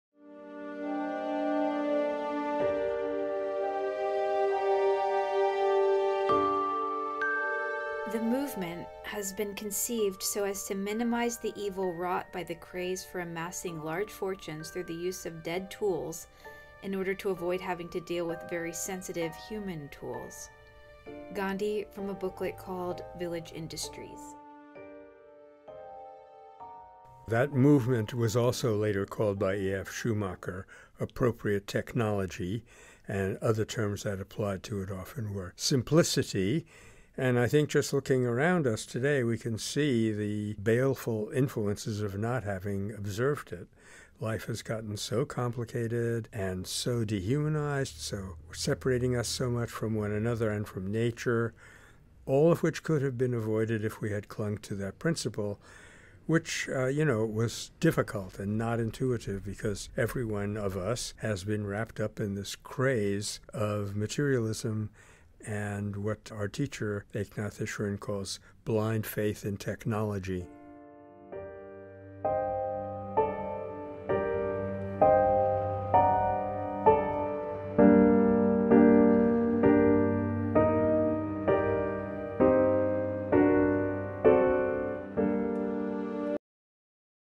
Nonviolence Daily | Day 195
The movement has been conceived so as to minimize the evil wrought by the craze for amassing large fortunes through the use of dead tools in order to avoid having to deal with very sensitive human tools.
-Gandhi, Village Industries